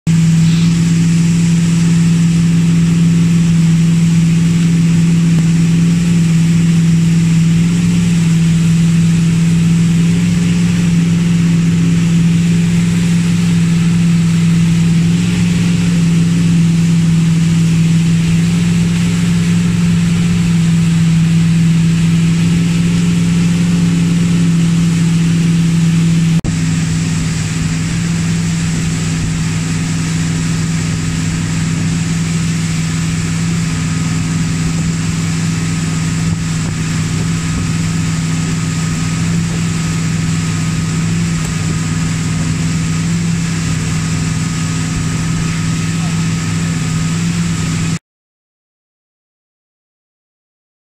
2018-01-10: Aaliyah Yoong Hanifah, 14, training 8290pts trick runs for 1st half 2018
Targets for 2018:
March Moomba - 7810 points
May US Masters - 8290 points
August Junior Worlds - 8490 points
Setting reasonably high but realistic tricks targets despite training for slalom and jump too. Matter of mind over matter.